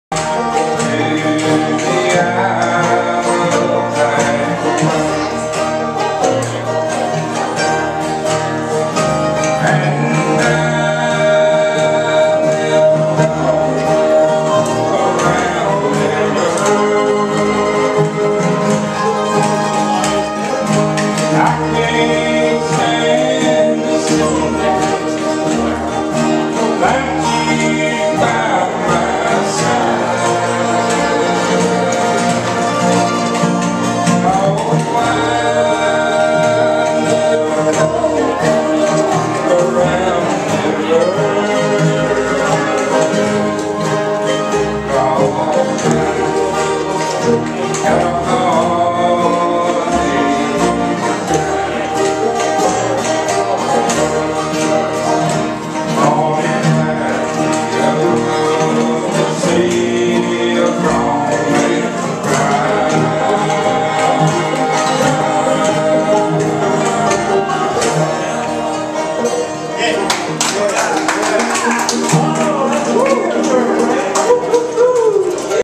Breazeals Bluegrass on 178 N.of Anderson,SC Inside pictures of performers.
Recorded on August 27, 2011 by Jimmy Gilstrap